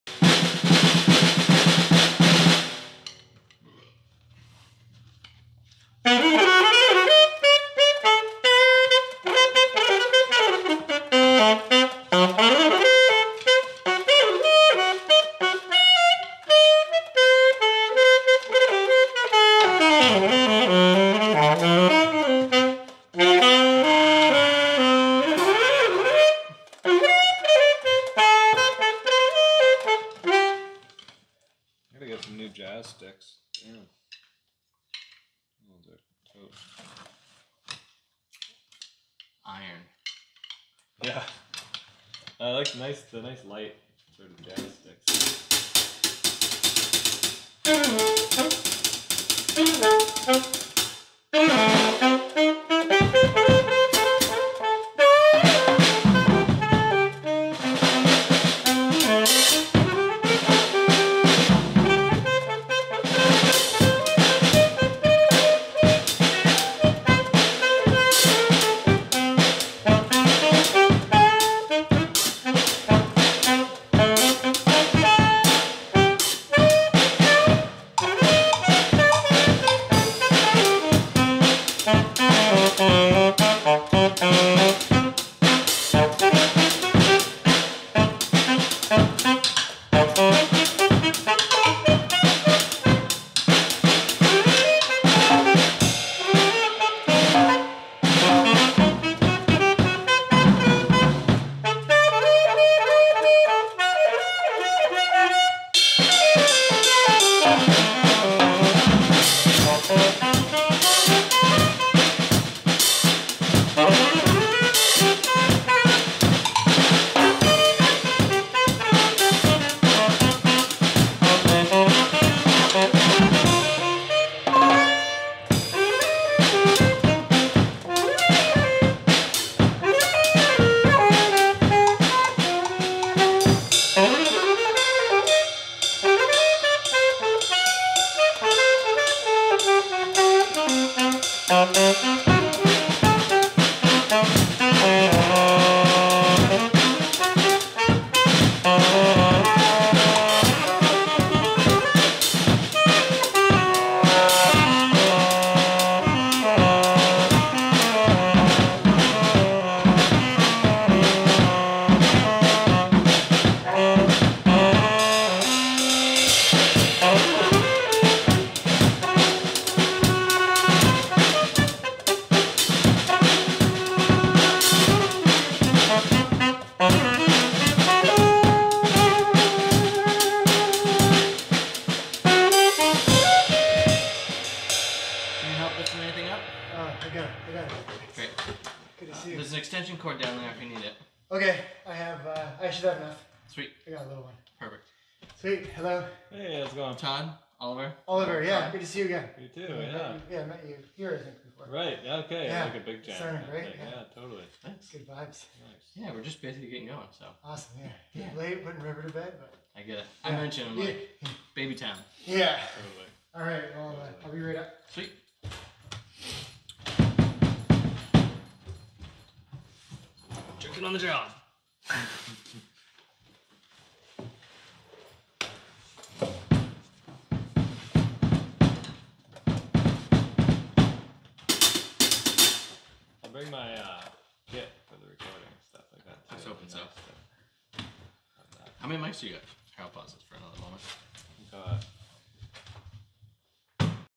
July 12th 2023 Music Lab A
July 12th 2023 MusicLab
Oliver Hodswood on Drums
Todd Barisky on Bass
Reed Richard on Sax